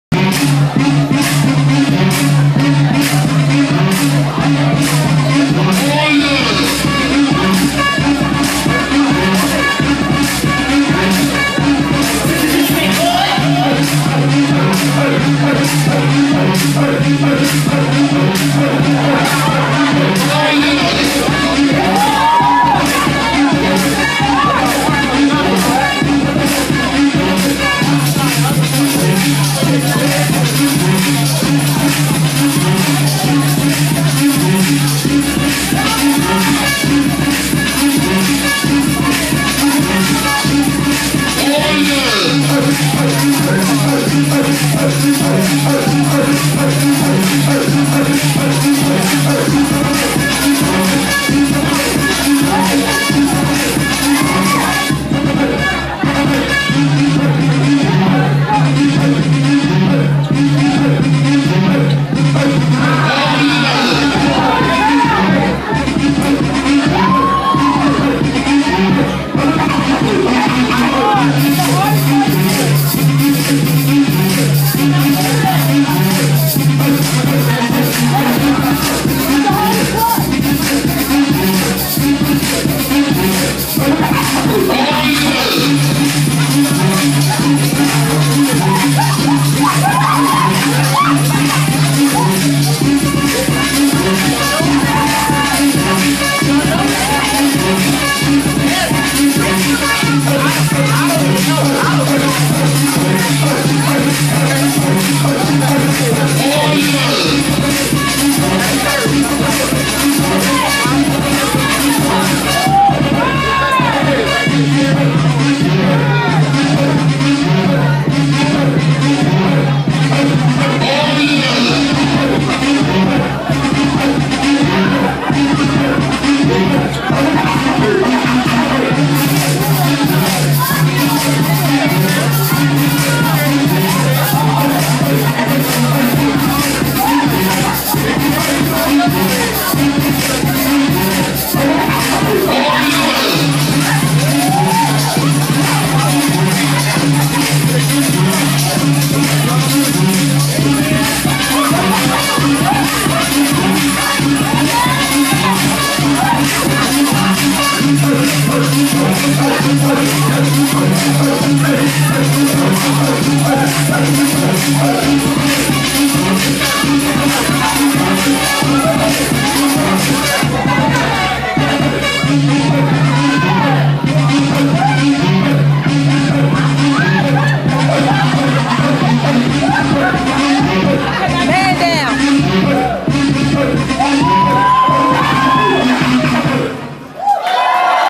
Intensity Line Dance
Line Dance Family Brunch - May 2013
Created by Perry, Michelle & Jerome